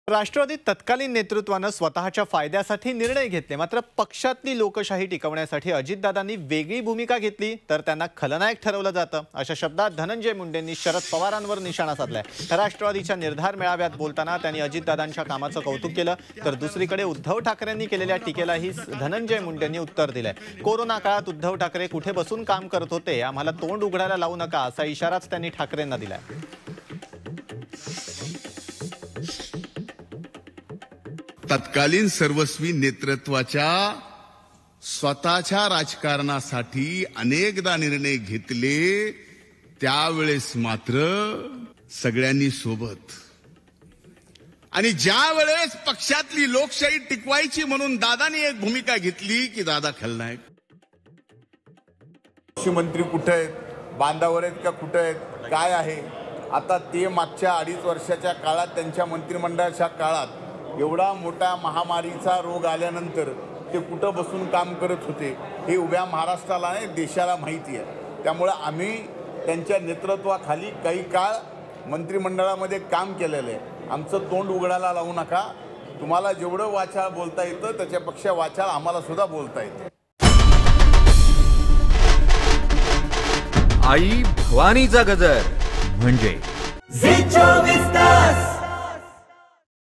Ajit Pawar | "अजितदादांनी खलनायक ठरवलं जातंय", धनंजय मुंडेंचा शरद पवारांवर निशाणा
The then leadership in NCP took decisions for their own benefit. But Dhananjay Munde has targeted Sharad Pawar by saying that if Ajit Dada takes a different stand to maintain the democracy in the party, he will be considered a villain. Speaking at NCP Nirdhar Mela, he praised Ajit Dad's work. On the other hand, Dhananjay Munde has also replied to the criticism made by Uddhav Thackeray. Uddhav Thackeray was sitting and working during Corona period, he warned Thackeray not to make us open our mouths.

राष्ट्रवादीत तत्कालीन नेतृत्वानं स्वत:च्या फायद्यासाठी निर्णय घेतले. मात्र पक्षातली लोकशाही टिकवण्यासाठी अजितदादांनी वेगळी भूमिका घेतली तर त्यांना खलनायक ठरवलं जातंय अशा शब्दात धनंजय मुंडेंनी शरद पवारांवर निशाणा साधलाय. राष्ट्रवादीच्या निर्धार मेळाव्यात बोलताना त्यांनी अजितदादांच्या कामाचं कौतुक केलं. तर दुसरीकडे उद्धव ठाकरेंनी केलेल्या टीकेलाही धनंजय मुंडेंनी उत्तर दिलंय. कोरोना काळात उद्धव ठाकरे कुठं बसून काम करत होते, आम्हाला तोंड उघडायला लावू नका असा इशाराच त्यांनी ठाकरेंना दिलाय.